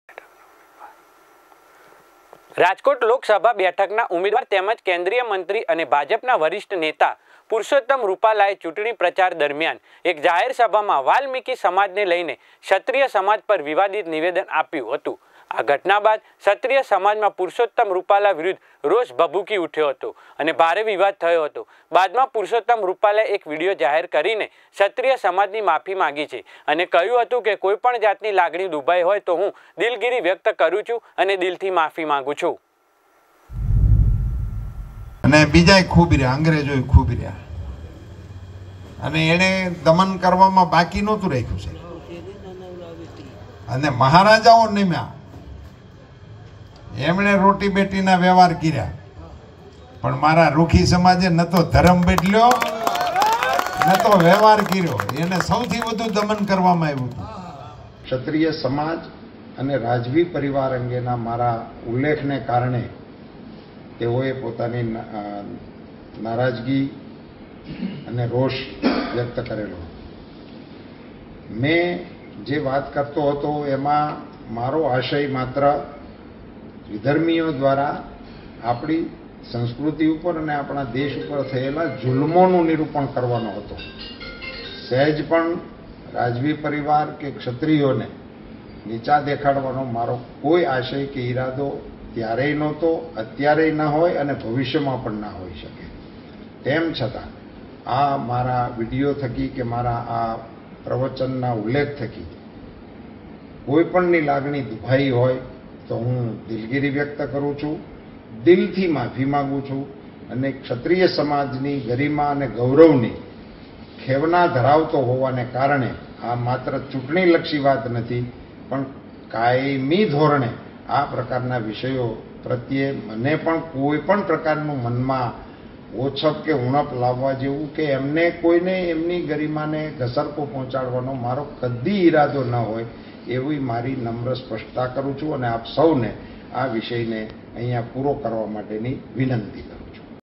પુરશોતમ રૂપાલા માફી માંગી | VR LIVE
પરશોત્તમ રૂપાલાનું ક્ષત્રિય સમાજ પર વિવાદિત નિવેદન
ક્ષત્રીય સમાજ નારાજ થતા માંગી માફી
પરશોત્તમ રૂપાલાએ ક્ષત્રિય સમાજની માફી માંગી
ક્ષત્રિયોને નીચા દેખાડવાનો કોઈ આશય ન હતો: રૂપાલા
-News Summary